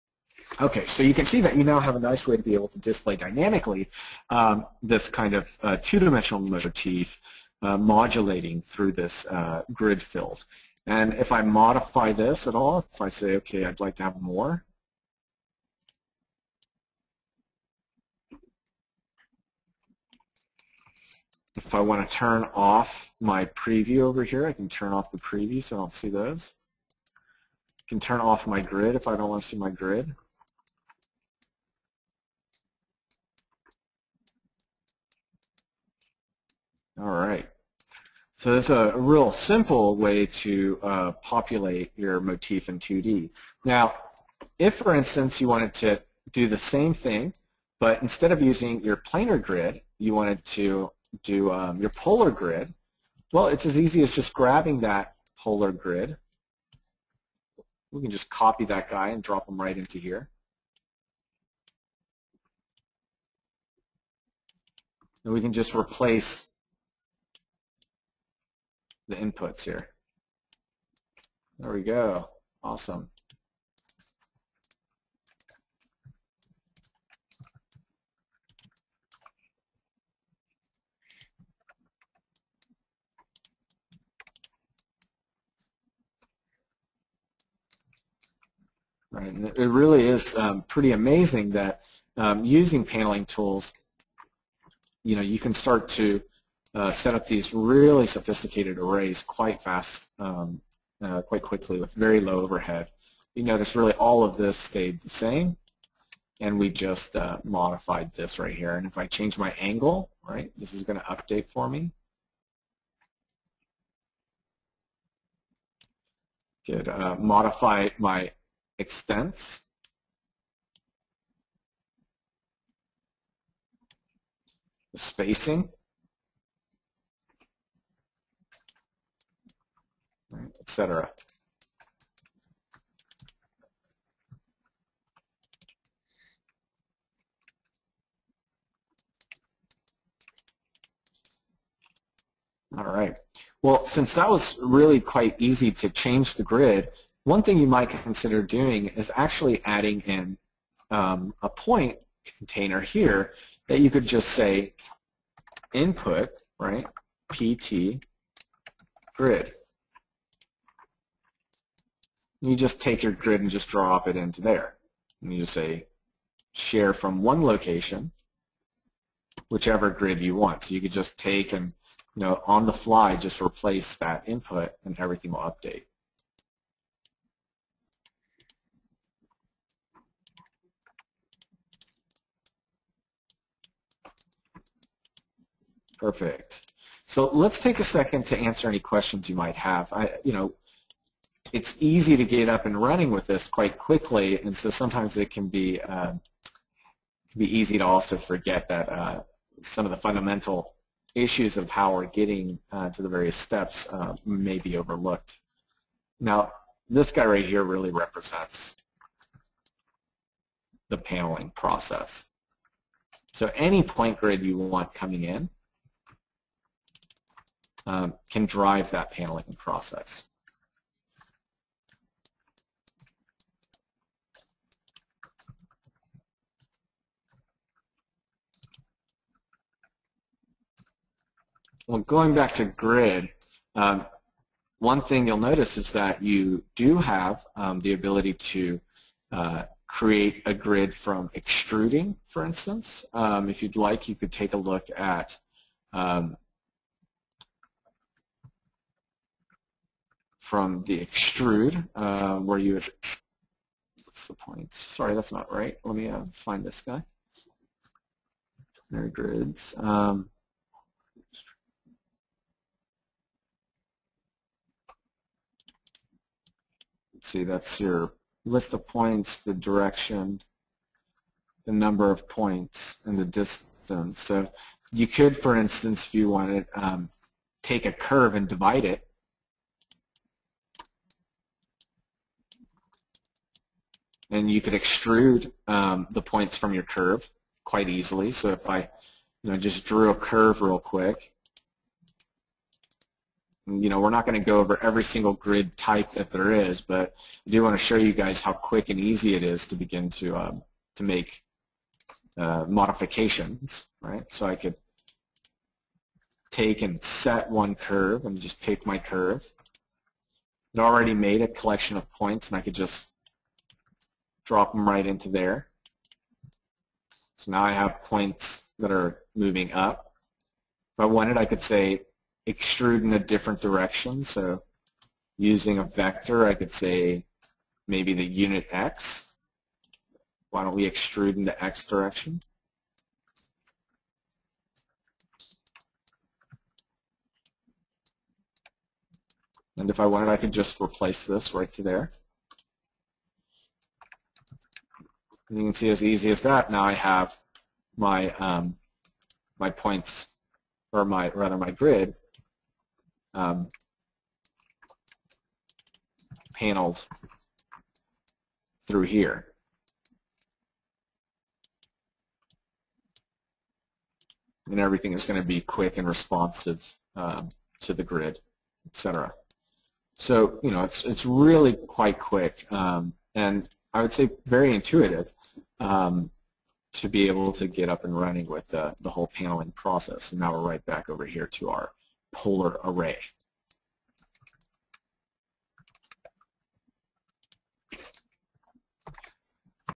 Parametric Pleating 11 | Paneling a 2D Motif Cont'd
Students will learn intuitive techniques, inspired by paper folding, for the design, analysis, and fabrication of complex surfaces. Additionally, the principles and rationale behind NURBS will be covered, shedding light on how this particular type of geometry may impact the various design decisions you will face when paneling surfaces.